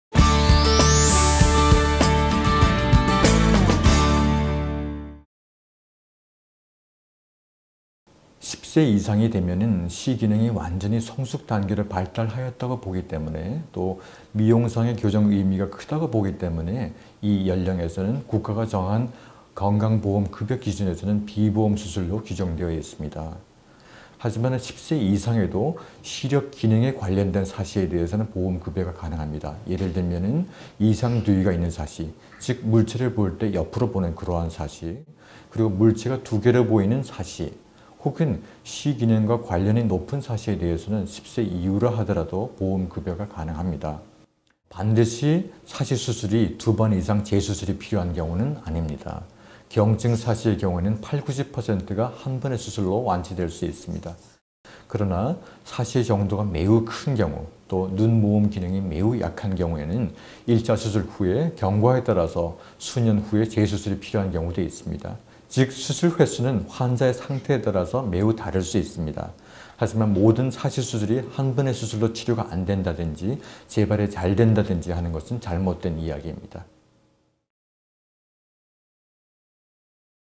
10세 이상의 소아 사시수술과 보험급여 가능 여부 [AMC 병법]
Q. 10세 이상이면 사시 수술이 왜 비보험인가요? 수술은 한 번만 하면 되나요?

10세 이상이 되면 시 기능이 완전히 성숙 단계로 발달하였다고 보고 미용상의 교정 의미가 크다고 보기 때문에
이 나이에서는 국가가 정한 건강보험 급여 기준에서는 비보험 수술로 규정되어 있습니다
하지만 10세 이상에도 시력 기능에 관련된 사시에 대해서는 보험 급여가 가능합니다
예를 들면은 이상두위가 있는 사시, 즉 물체를 볼 때 옆으로 보는 그러한 사시 그리고 물체가 두 개로 보이는 사시(복시)
혹은 시 기능과 관련이 높은 사시에 대해서는 10세 이후라 하더라도 보험급여가 가능합니다
반드시 사시 수술이 두 번 이상 재수술이 필요한 경우는 아닙니다
경증 사시의 경우에는 80~90%가 한 번의 수술로 완치될 수 있습니다
그러나 사시의 정도가 매우 큰 경우 또 눈 모음 기능이 매우 약한 경우에는 1차 수술 후에 경과에 따라서
수년 후에 재수술이 필요한 경우도 있습니다
즉 수술 횟수는 환자의 상태에 따라서 매우 다를 수 있습니다
하지만 모든 사시 수술이 한 번의 수술로 치료가 안 된다든지 재발이 잘 된다든지 하는 것은 잘못된 이야기입니다

임현택 교수 / 서울아산병원 안과